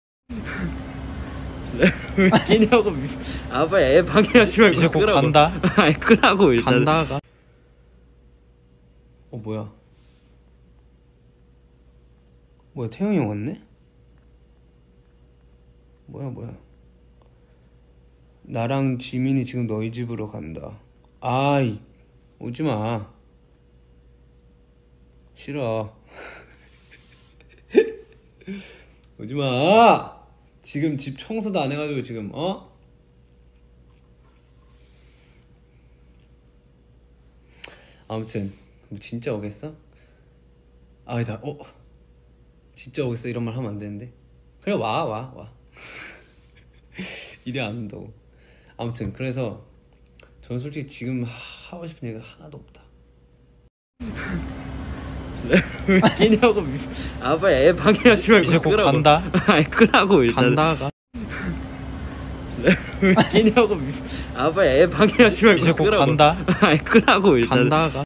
Live🔴 BTS Taehyung & Jimin On their Way To Jungkook's House BTS V and Jimin Weverse Live 20231103
Taehyung  V on Weverse Live Today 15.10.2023 (Eng Sub)  @B4Bts-cy1in

Taehyung  V on Weverse Live Today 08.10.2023 (Eng Sub)  @B4Bts-cy1in